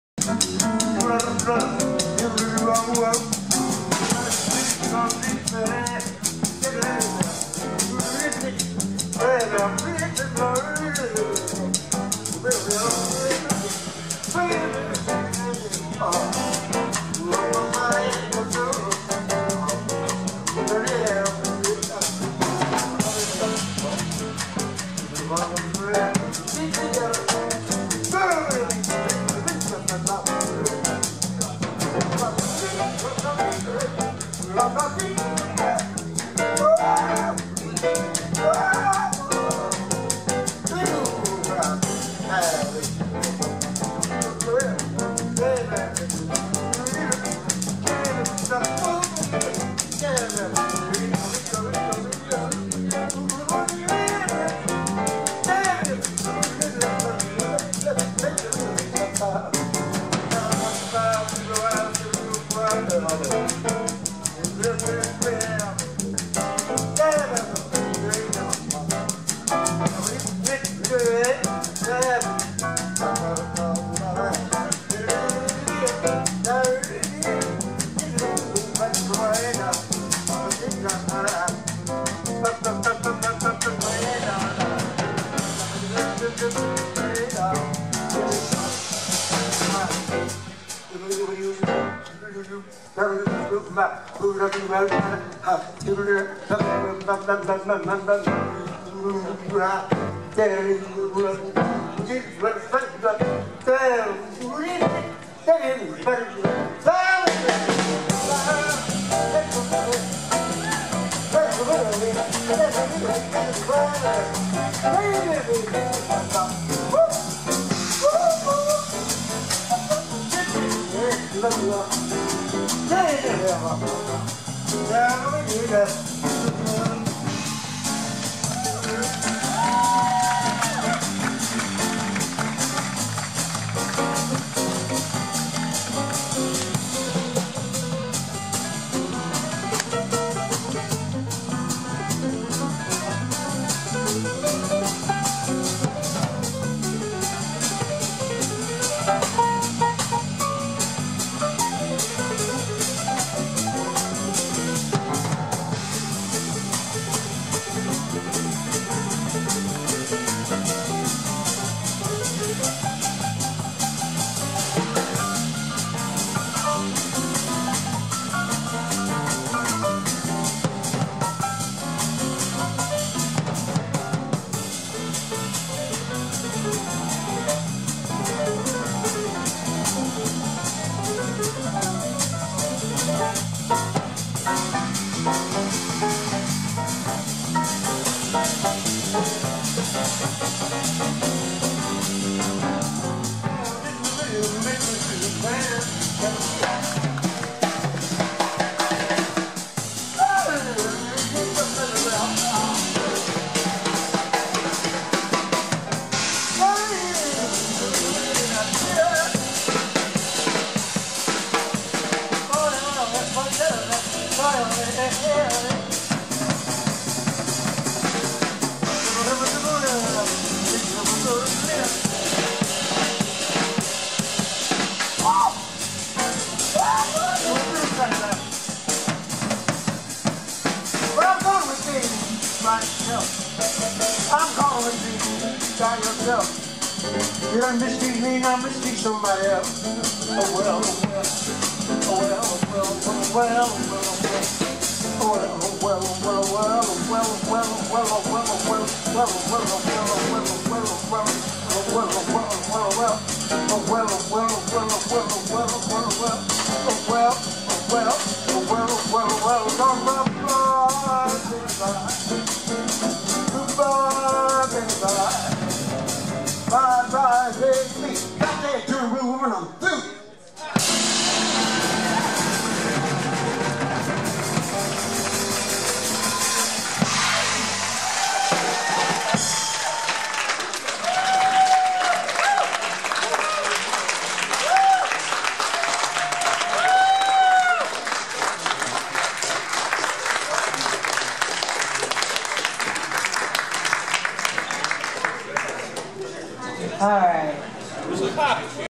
Jazz on the Maumee #9--Jon Hendricks & Swingmania
Jon Hendricks & Swingmania providing a tasty, upbeat, musical cocktail. Video by Chuck/CamericaTV.